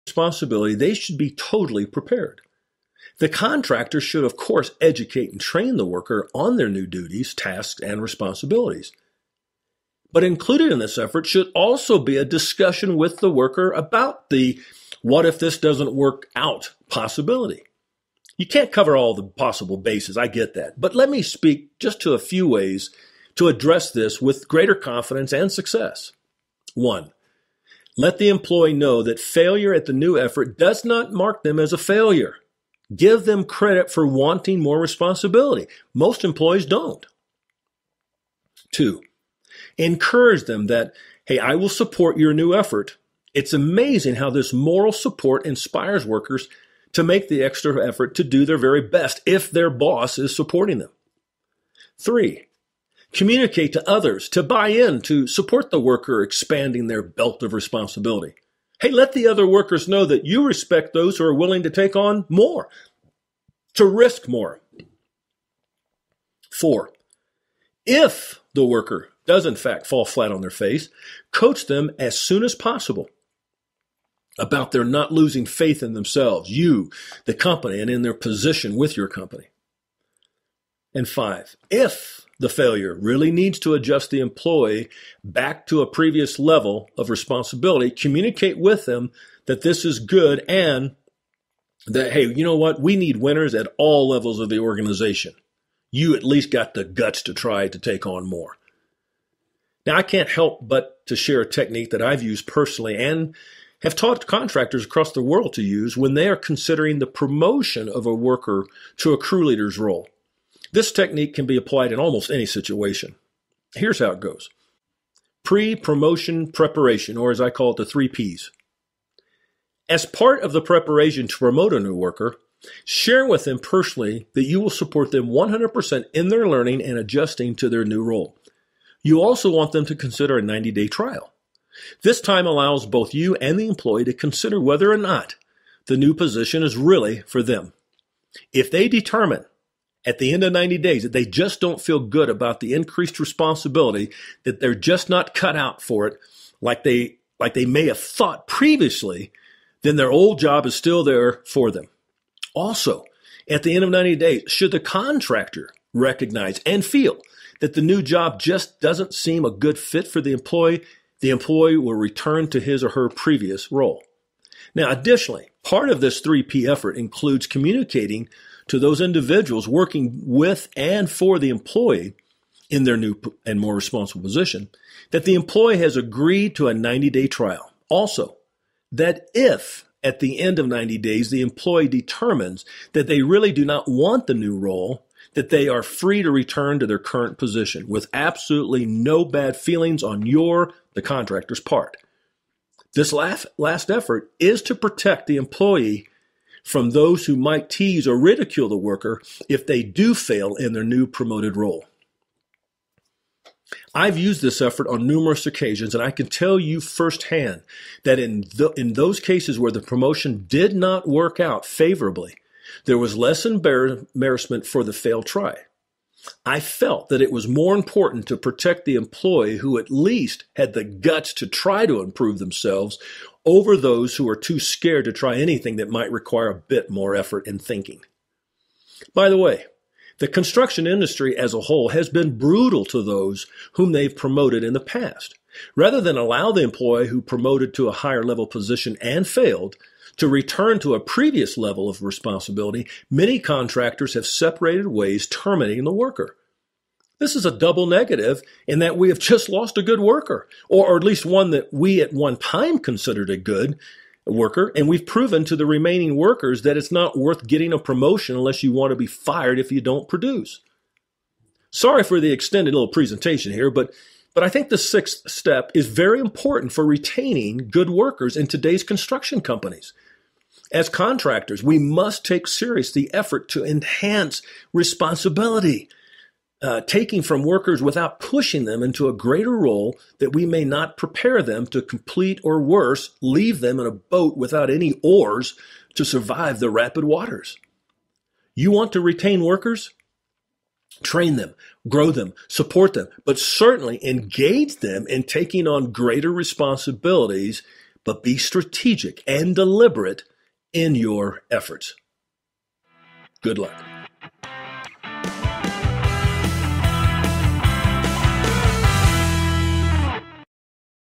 7 Steps - Responsiblity Enhancement (part 2 of 2)
When a new worker has come on board there is an expectation that it will take a period of time for the employee to learn their job, learn about the company’s work culture, work processes, and to learn about their new co-workers.

In this episode, learn how you can increase your employee's responsibility over time to enhance your worker retention.